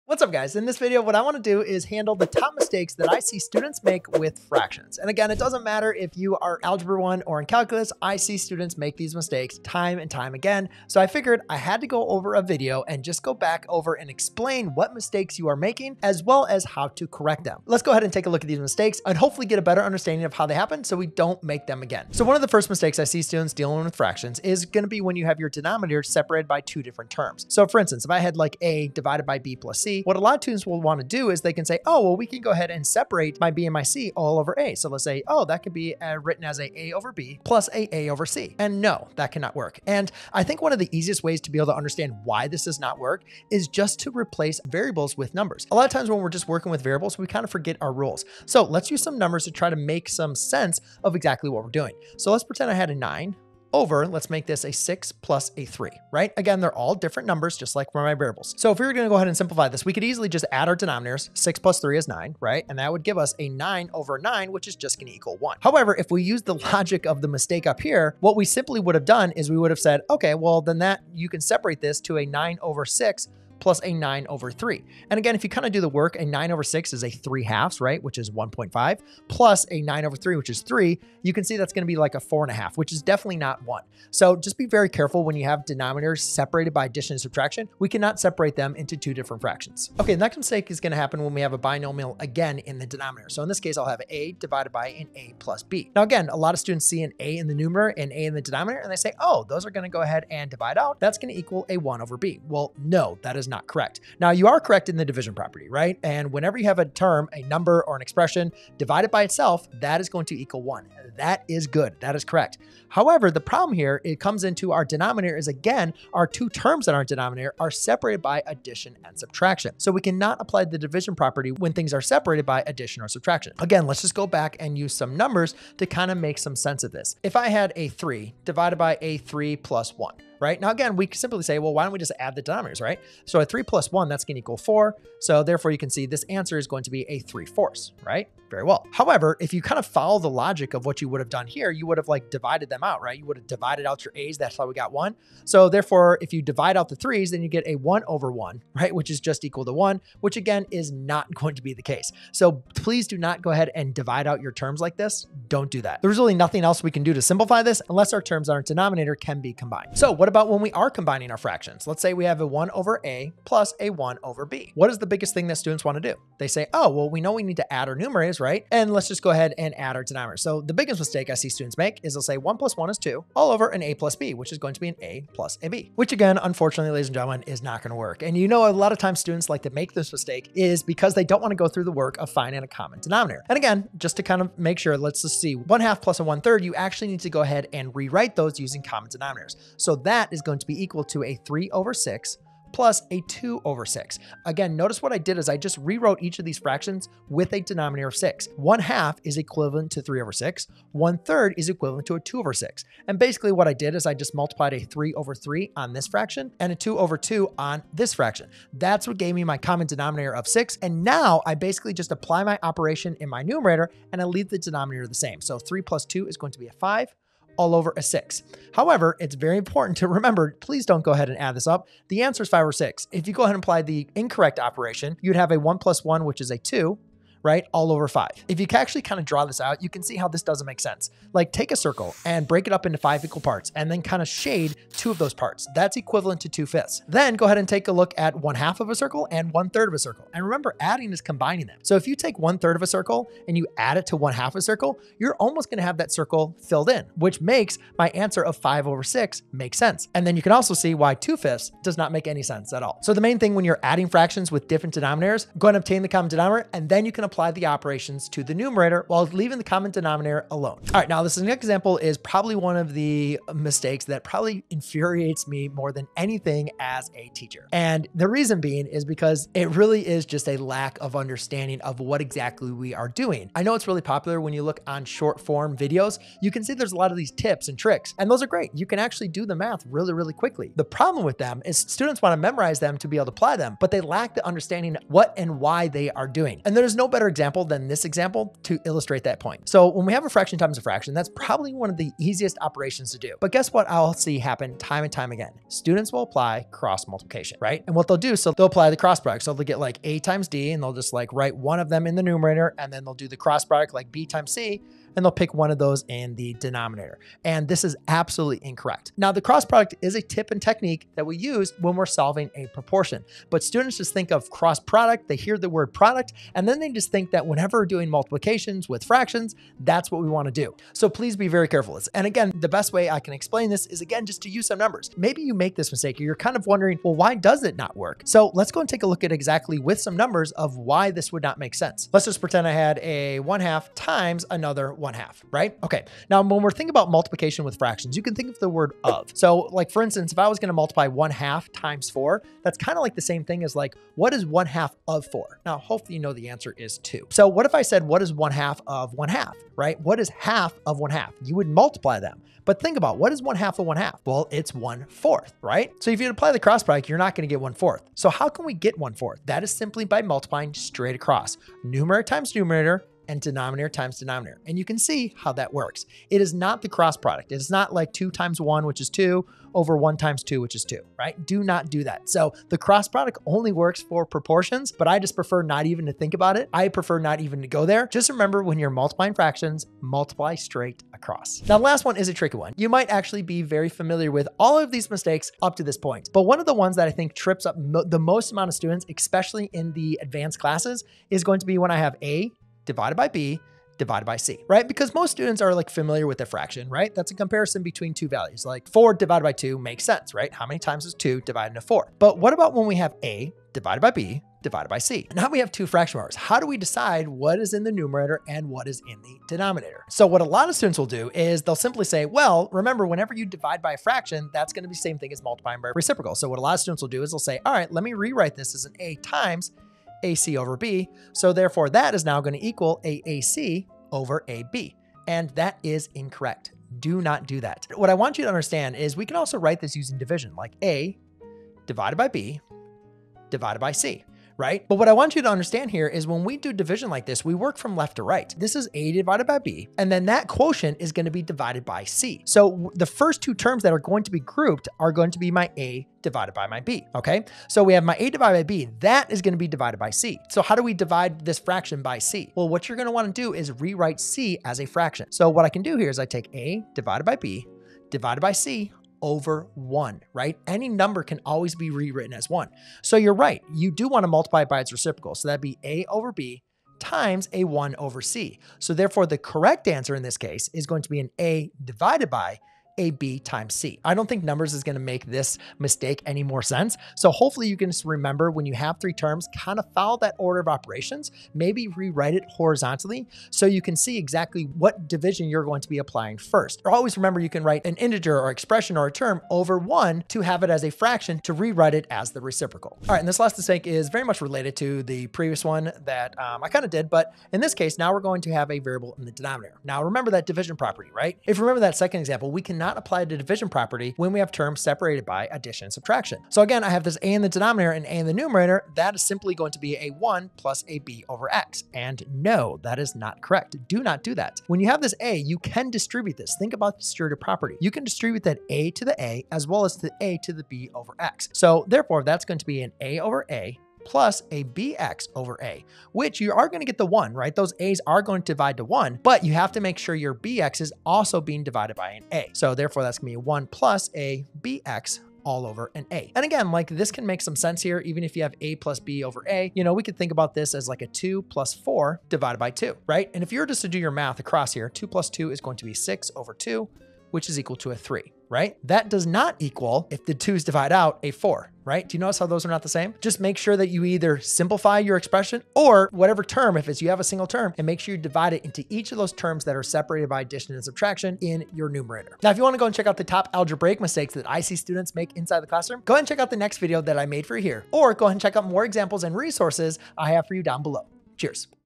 Worst Algebra Mistakes Students Make With Fractions
We all know that fractions can be difficult. So what are the most common errors I see students make when they using fractions. Well in this video we will explore just that.